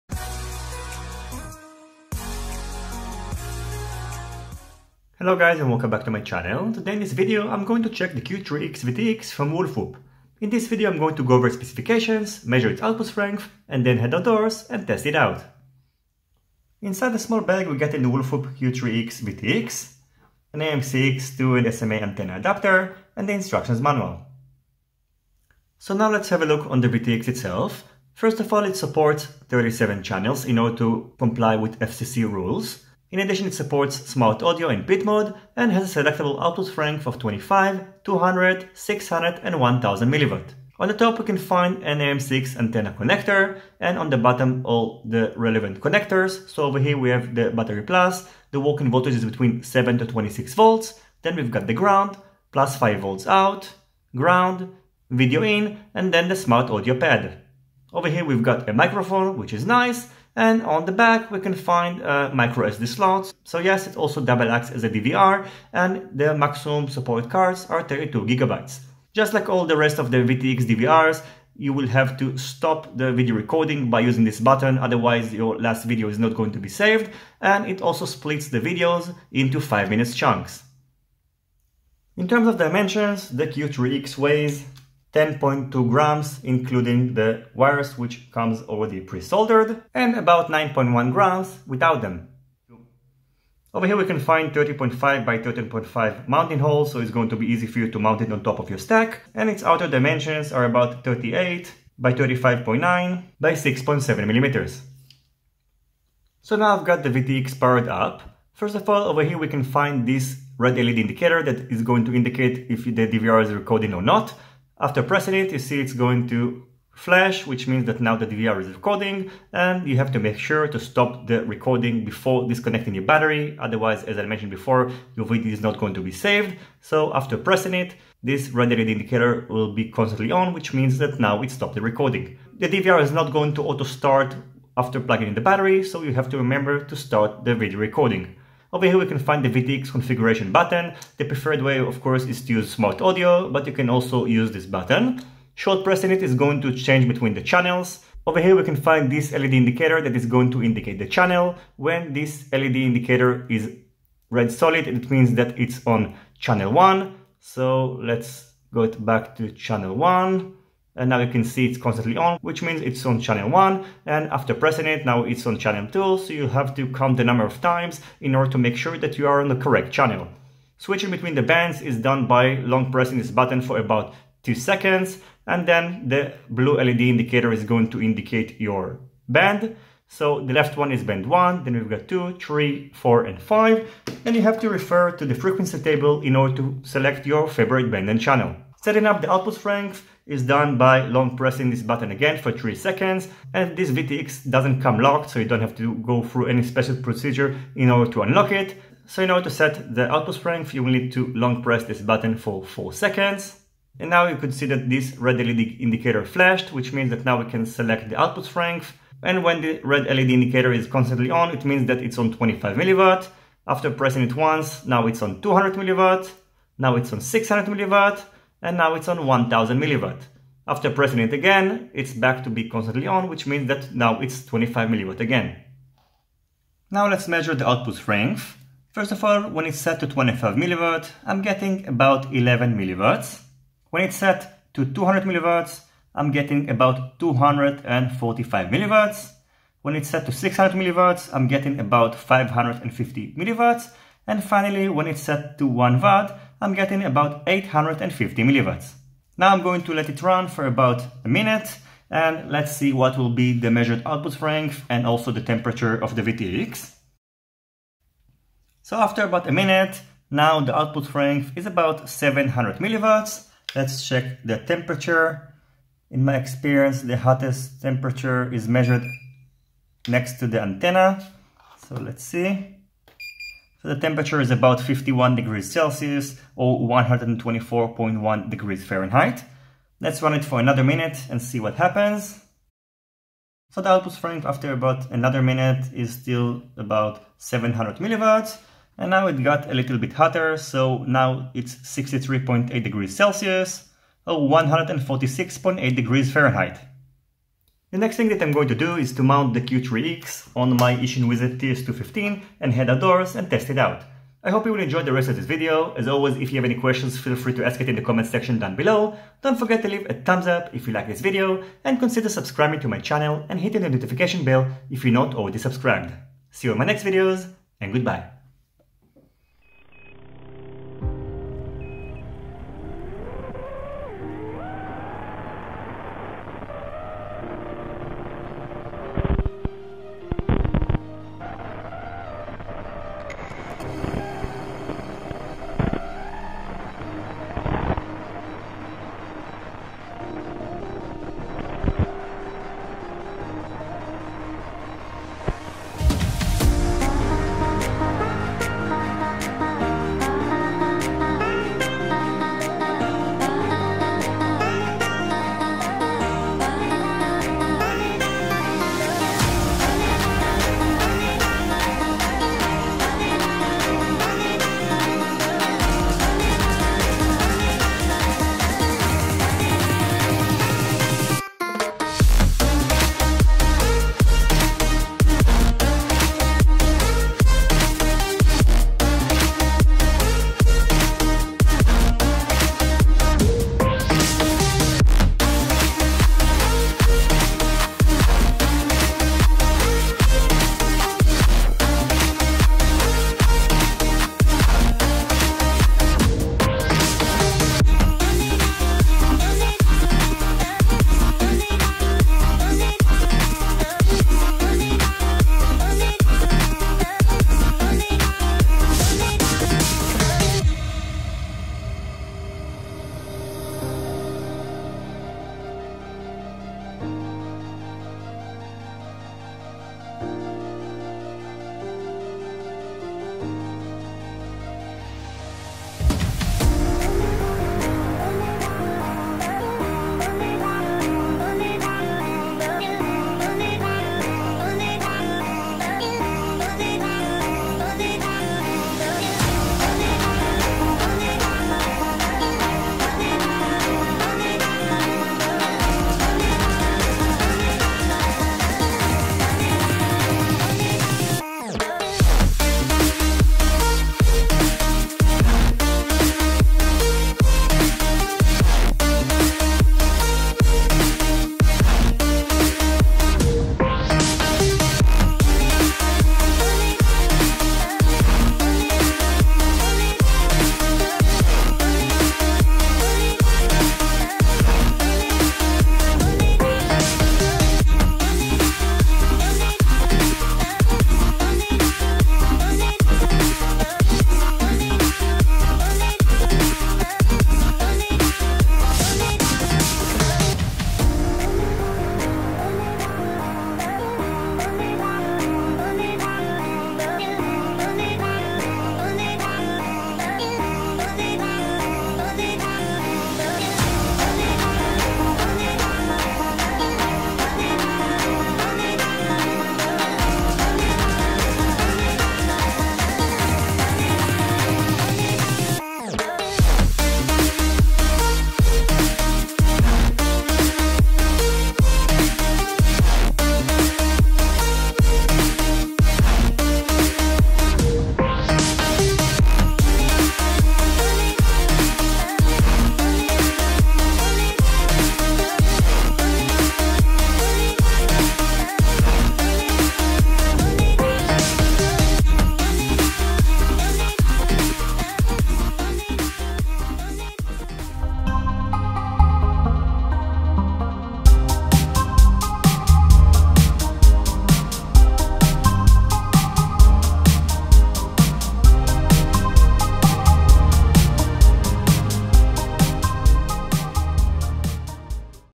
Wolfwhoop Q3-X VTX With Build in DVR
╠╗║╚╝║║╠╗║╚╣║║║║║═╣ I Really Appreciate Your Support!

Description:

Wolfwhoop Q3-X 5.8GHz 0.01/25/200/600/1000mW Switchable FPV Video Transmitter Smart Audio Build in DVR

Specifications:
Operating Voltage: 7 - 26V
Power supply: 5V output for camera
Output power:25/200/600/1000mw
Antenna connector: MMCX
Video Format: NTSC/PAL
Port connector: solder pads
Dimensions: 36(W)x36(D)mm, 30.5x30.5mm(stackable hole)
Weight:8.8g(excluding antenna)

Package included:
1 X Q3-X VTX(with cable)
1 X User Manual
1 X MMCX to SMA Female antenna cable